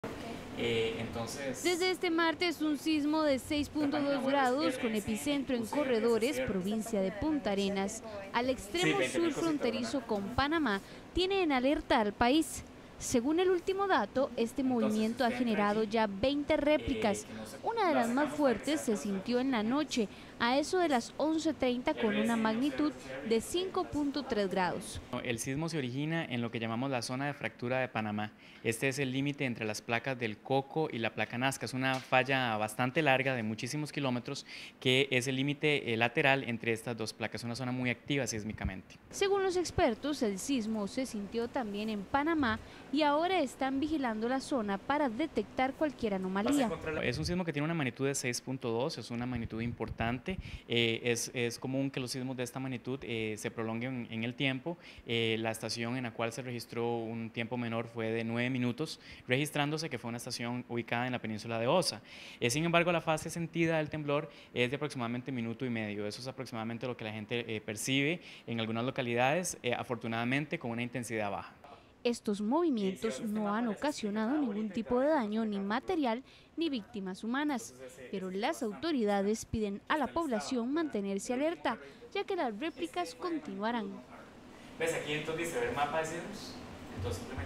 Cerca de 20 réplicas se han sentido después del sismo del pasado martes
El sismo que sacudió al país este martes ya ha presentado 20 replicas según las Red Sismológica Nacional, la más fuerte sentida anoche fue de 5.3 grados.